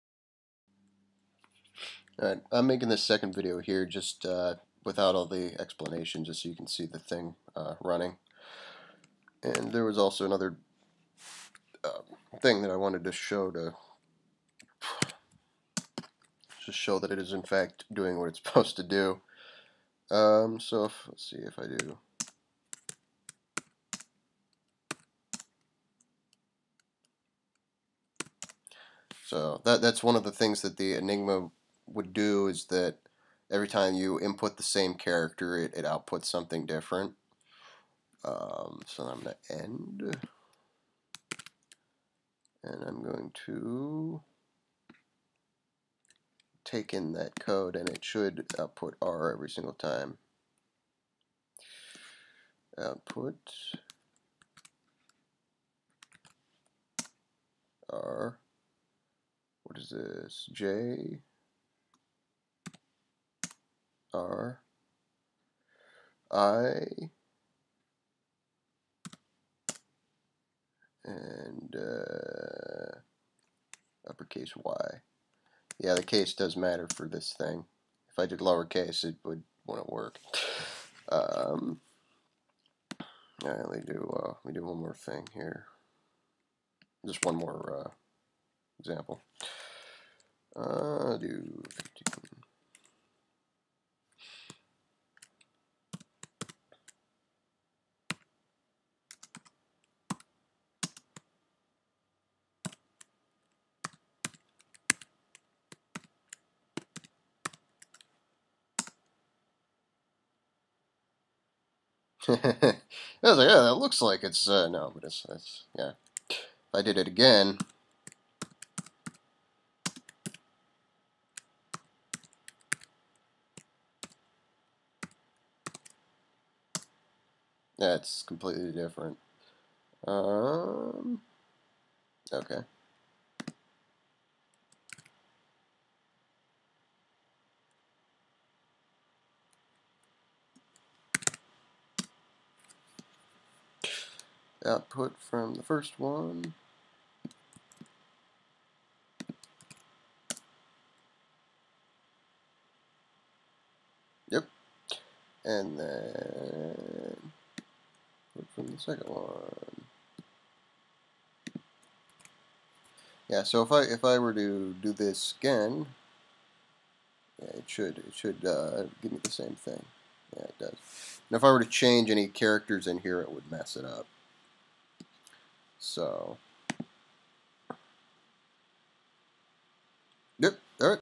Java Enigma Emulator (Quick Demo)
This is a brief demo of the Enigma emulator program I wrote in Java.